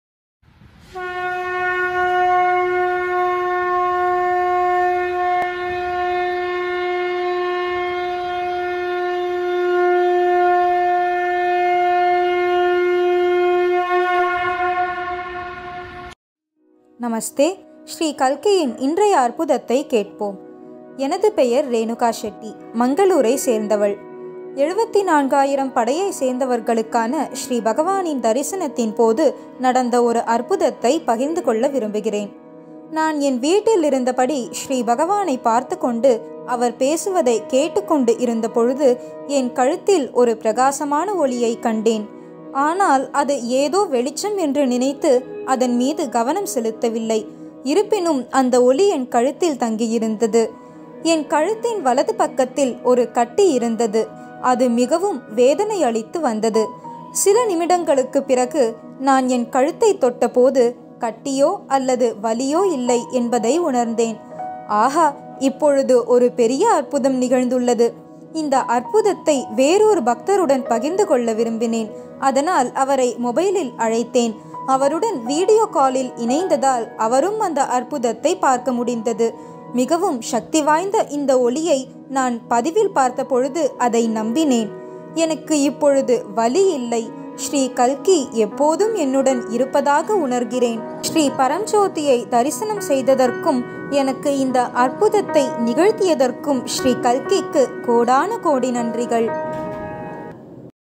என் கழுத்தில் வலி கொடுத்து வந்த ஒரு உள்கட்டி 7400 தீட்சை நிகழ்வின்போது மறைந்து போனது.
ஶ்ரீ கல்கி அற்புதங்கள்